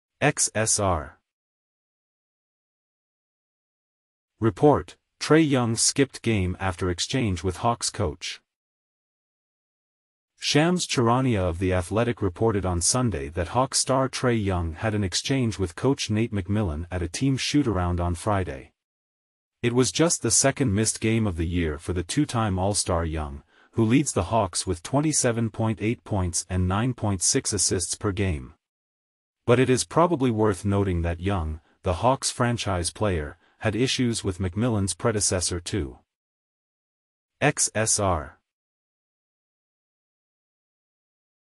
Report: Trae Young Skipped Game After Exchange With Hawks Coach
Report: Trae Young skipped game after exchange with Hawks coach. Shams Charania of The Athletic reported on Sunday that Hawks star Trae Young had an exchange with coach Nate McMillan at a team shootaround on Friday. It was just the second missed game of the year for the two-time All-Star Young, who leads the Hawks with 27.8 points and 9.6 assists per game. But it is probably worth noting that Young, the Hawks' franchise player, had issues with McMillan's predecessor too.